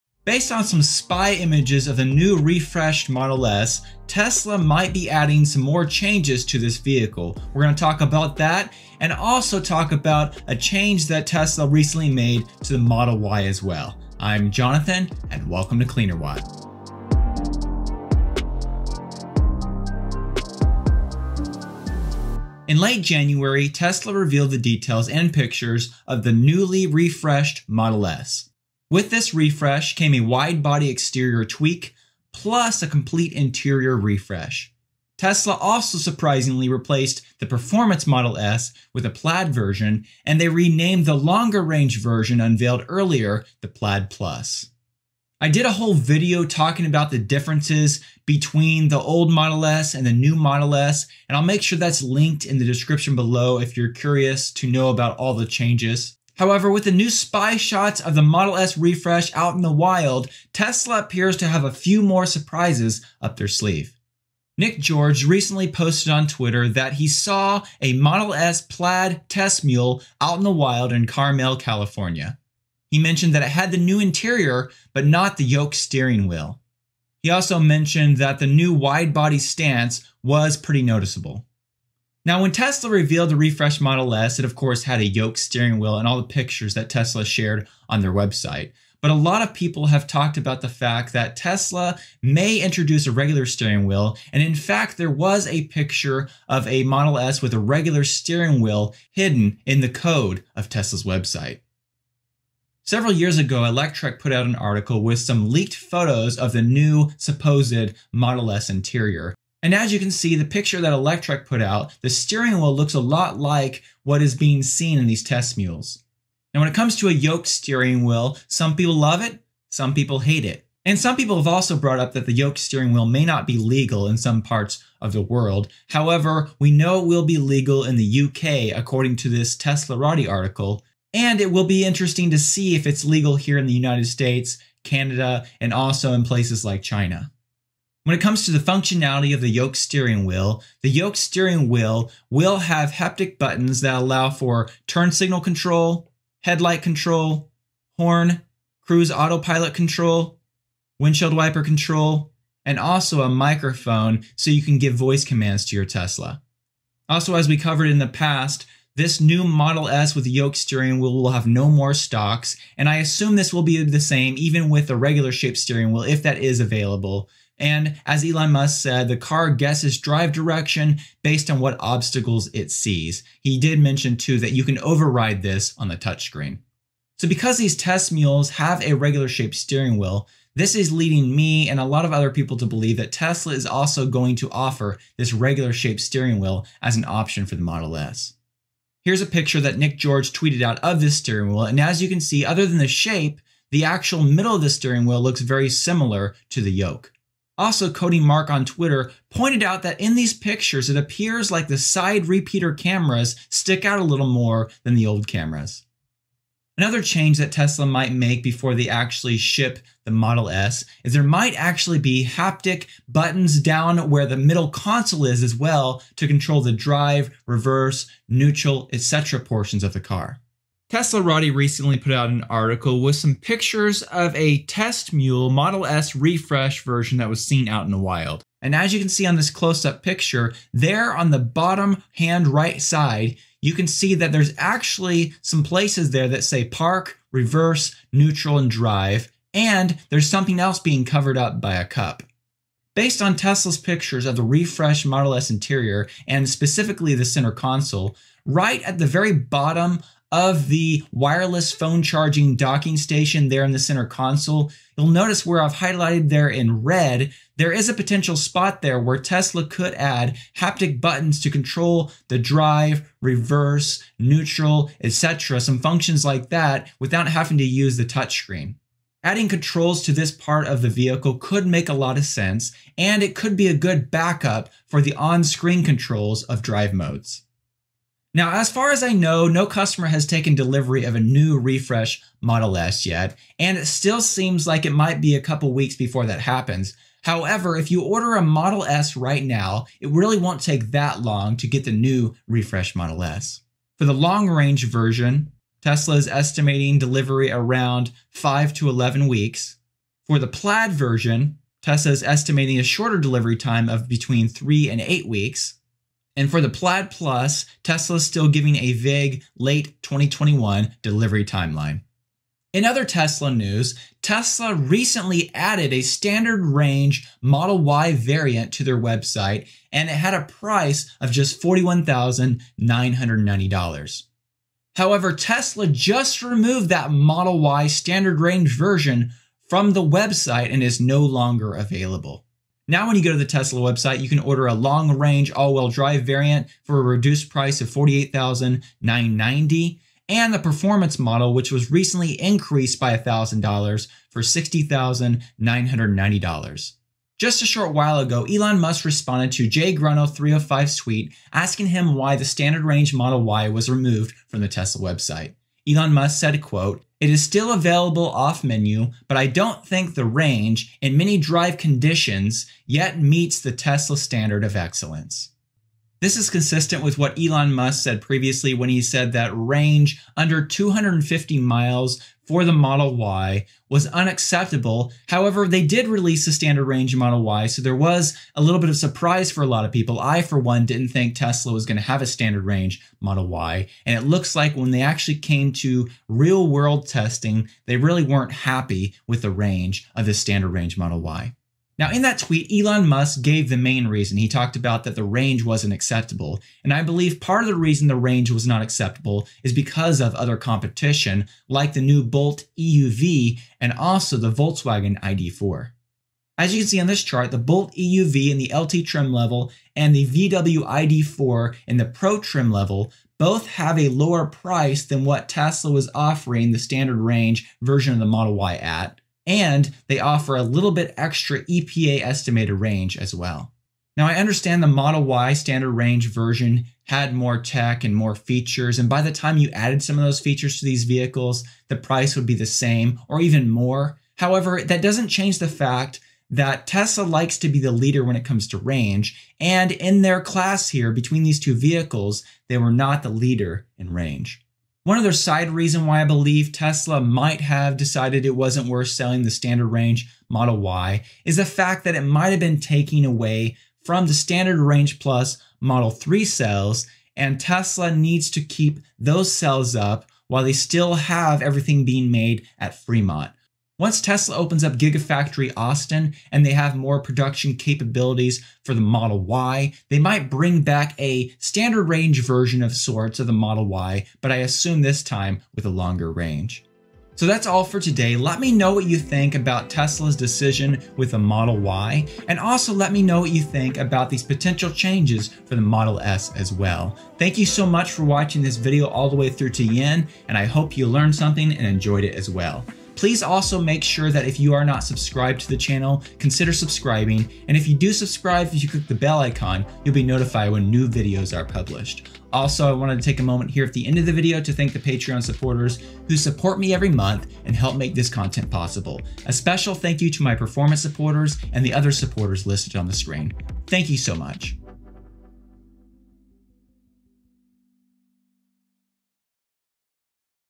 Tesla Model S and Model Y: More Changes Coming
In late January, Tesla revealed the details and pictures for the NEWLY REFRESHED Model S. However new spy shots of the Model S refresh are showing up online, and there appear to be a few more surprises coming. Also, Tesla has CANCELLED the Model Y SR version because the range wasn't up to Tesla's standards of excellence.

Support Cleanerwatt by:

2.

Image & Video Clip Sources:

1.  Tesla Media
2.  VW Media
3.  Chevrolet Media